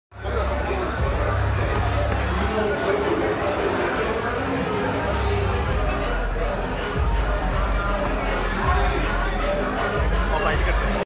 Viral... EXCLUSIVE Producer Vikas Gupta Birthday Bash at BARREL The GastroBar(2)
Producer Ekta Kapoor, Ajaz Khan and May More TV Stars are present on occasion of colourful night... PARTY